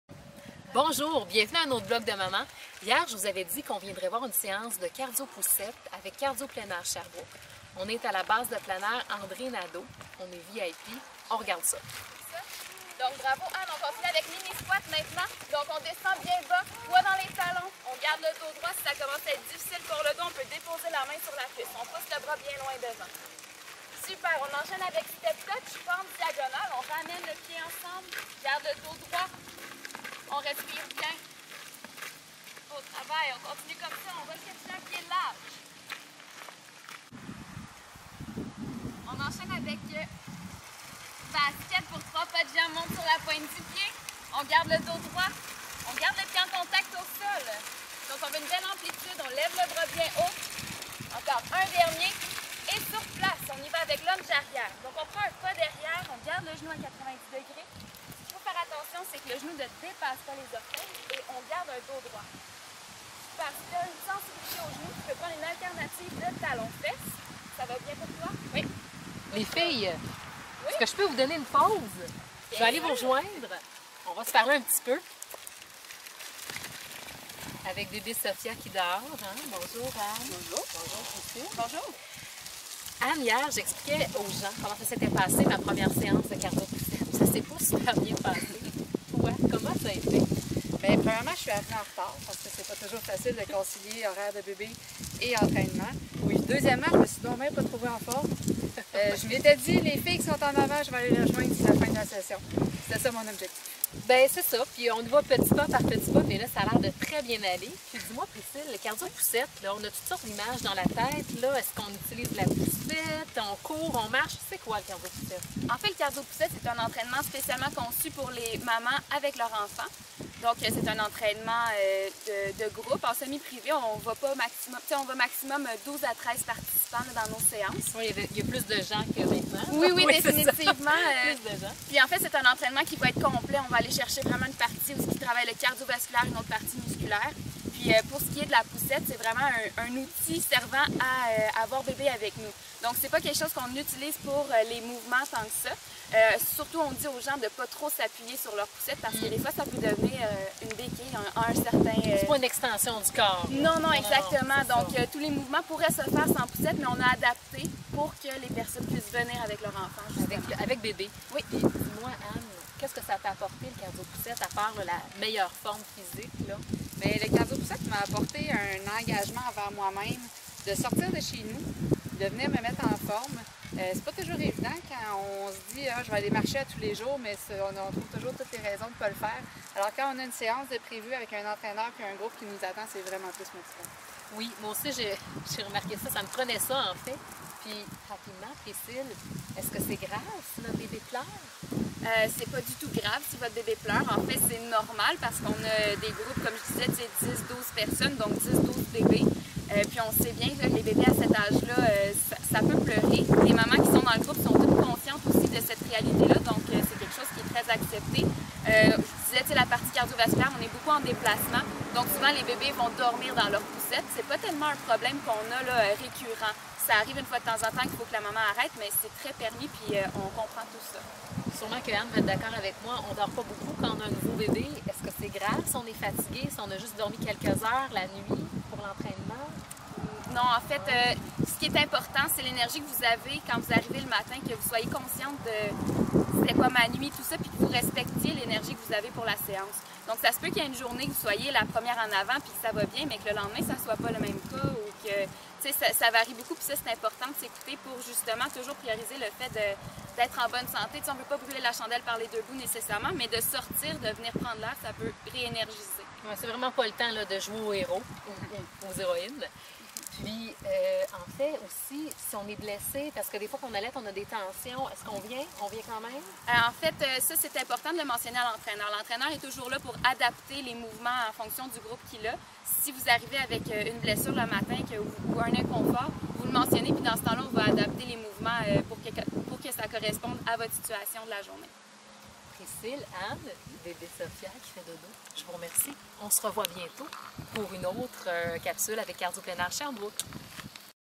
Cardio Plein Air: Cardio Poussette
Pascale Clavel du blogue unautrebloguedemaman.com teste pour vous le Cardio Poussette.